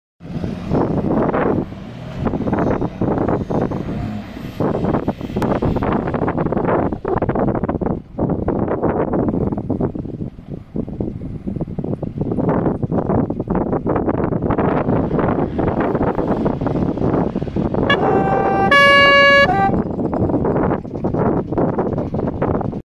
Range Rover Evoque Recovers 2 stuck Land Rover Defenders in one single go - LR Steve
This humble Evoque successfully recovered two defenders in one day. The first, being the one that's helping with this recovery, it got bogged trying to pull the Cummins powered monster out, so the Evoque came to it's rescue, pulled it out successfully, then using its help, dragged at an angle the Cummins powered beast that was stuck up to its chassis.

The Evoque in the video uses Active Driveline by GKN which is an advanced system enabling epic power distribution over the rear axle without the requirement of braking for traction control. This allows the backend of the Evoque to dig in, and power on through, even with 4 tonnes of defenders behind it.

PS - the first defender I believe was trying to see how quickly he could get stuck.